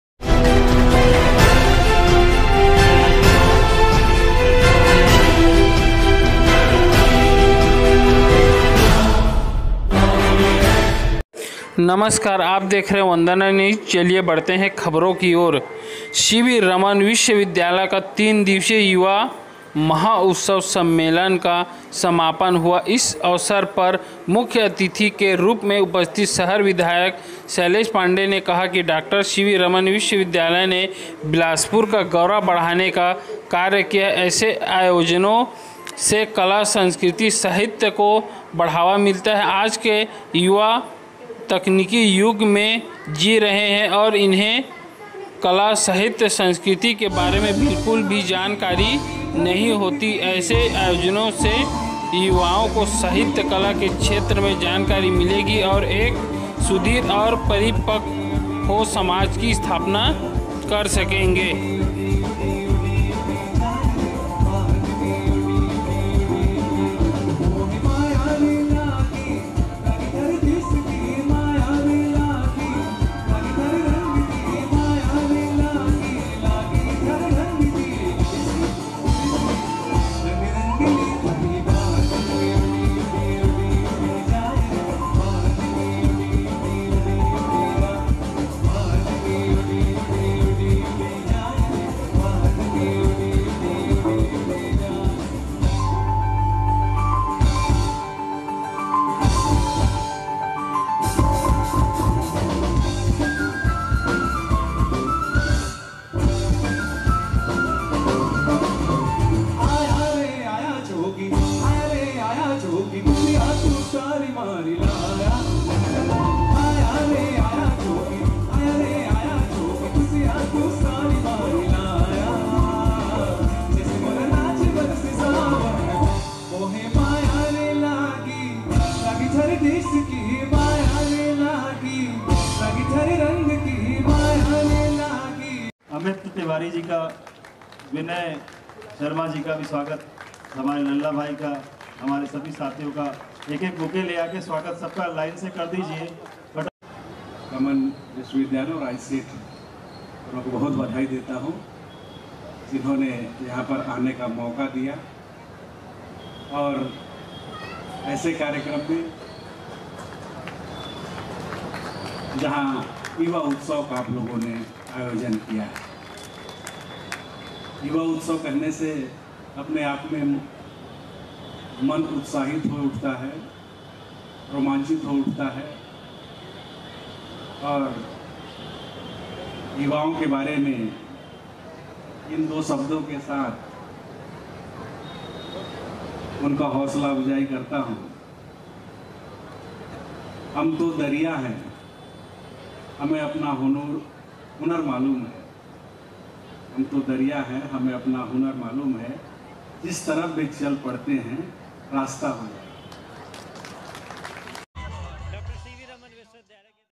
Vandana News Coverage of Day-3 of Yuva Utsav at Dr CV Raman University Bilaspur
Vandana News Coverage of Yuva Utsav at Dr CV Raman University Bilaspur!

or
Tagore International Literature and Arts Festival